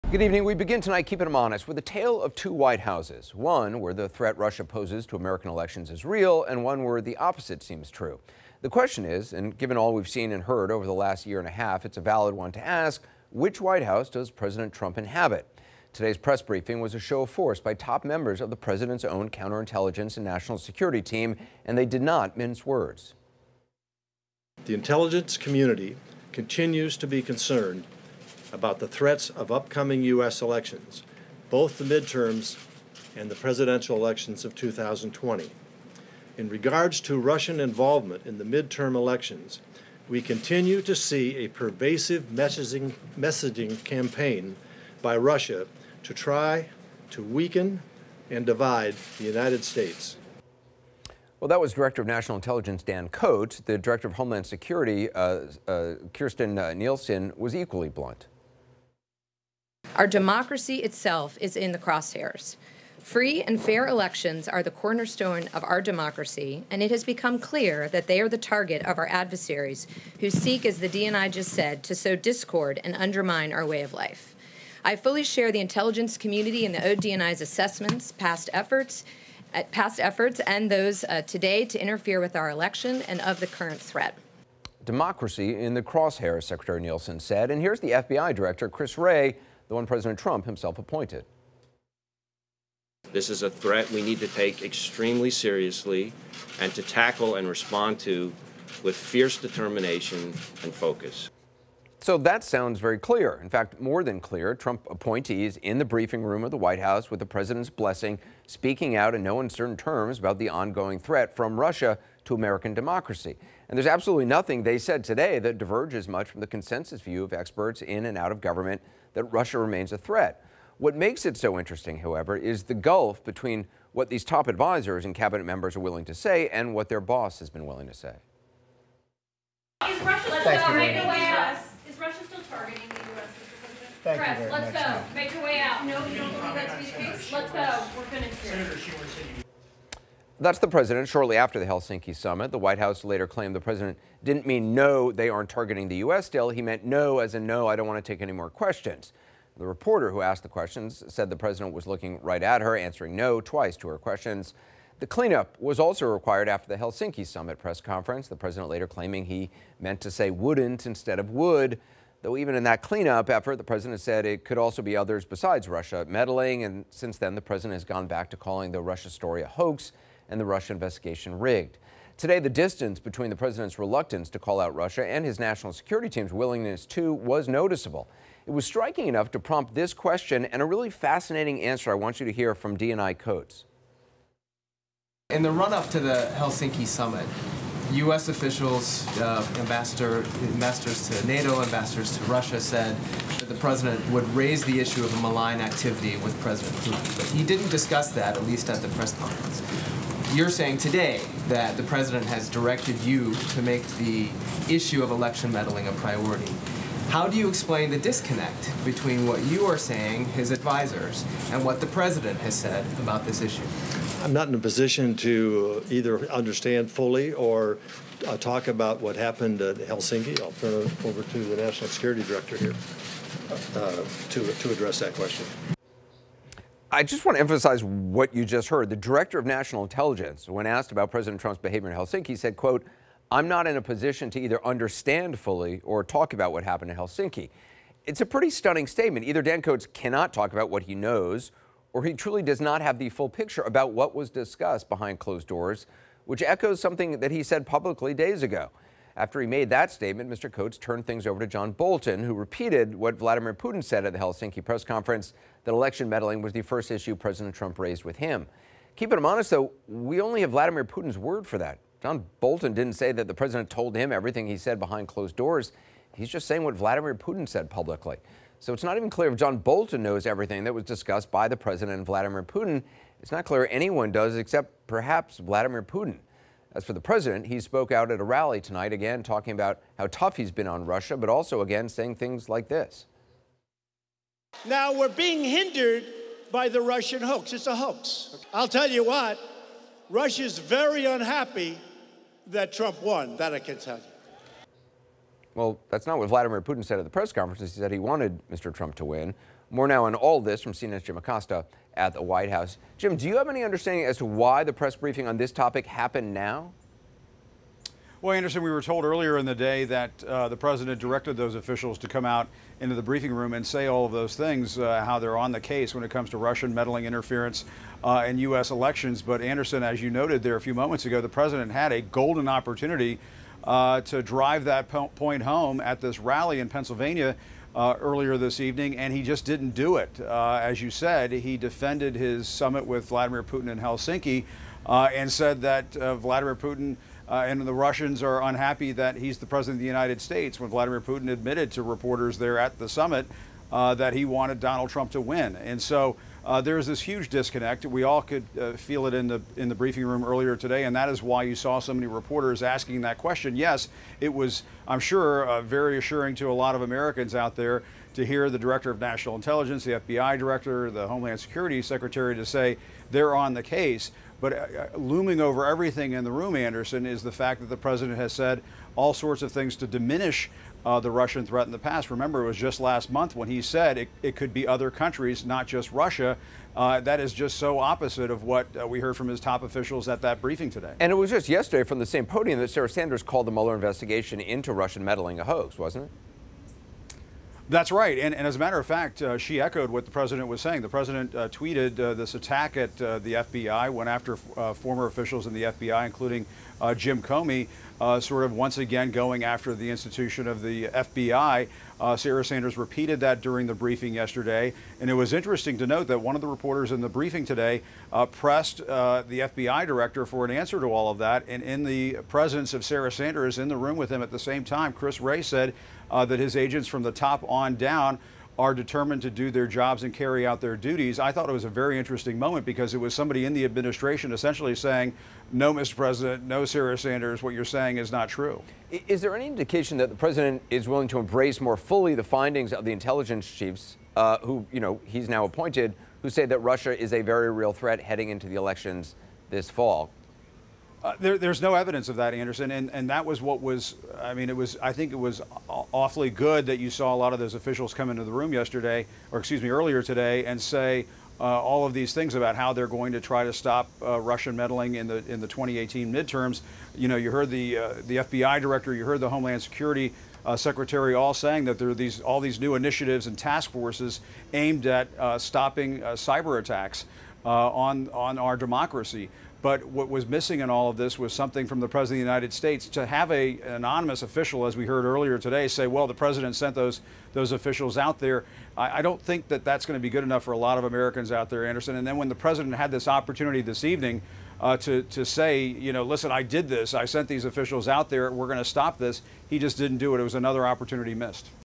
Anderson Cooper: This is a tale of two White Houses
A handful of top US national security officials said that Russia is continuing to pursue its efforts to interfere in the US political system and President Donald Trump has directed them to make countering election interference a top priority.